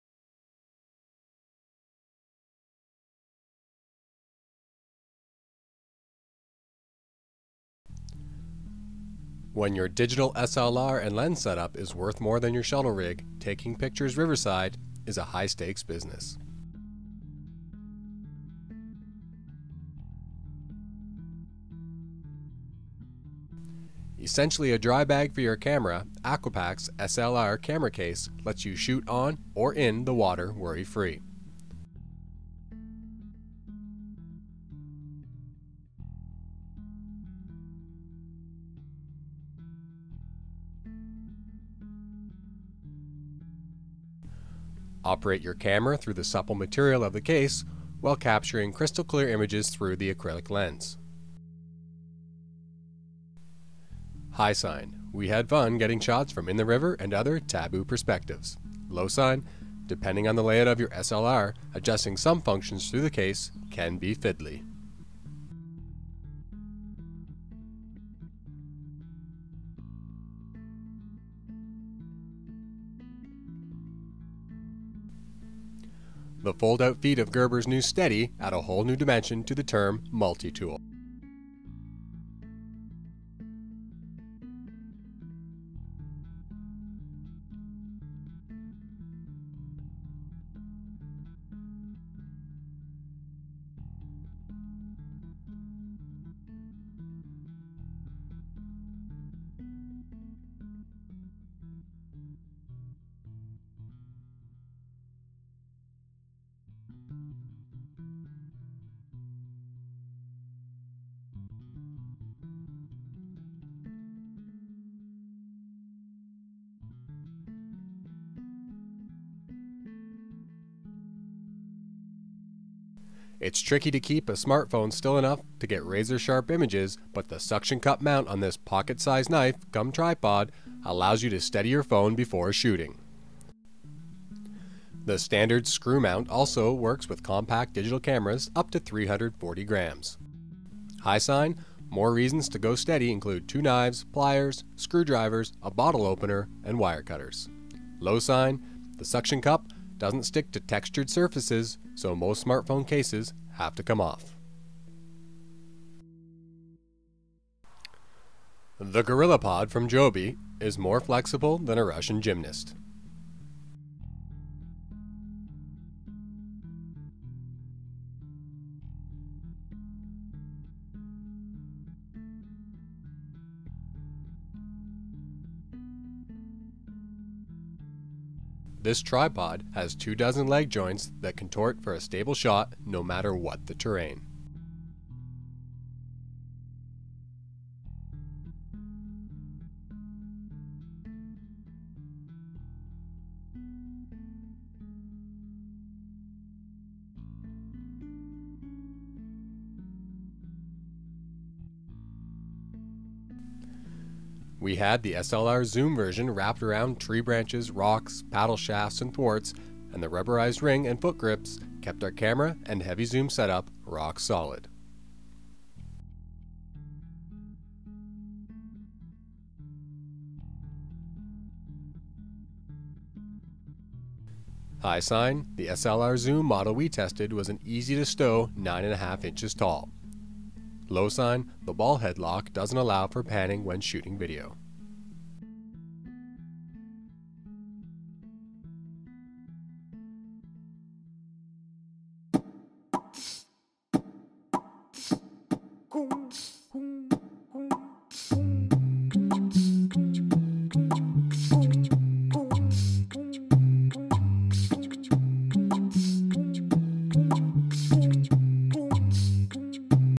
Camera Gear for the Adventure Photographer | Review | Rapid Magazine | Rapid Media
We check out a few pieces of gear that will make your photography sessions in and around the water easier and more fun. Joby and Gerber help keep things steady while an Aquapac drybag helps get your DSLR closer to the action.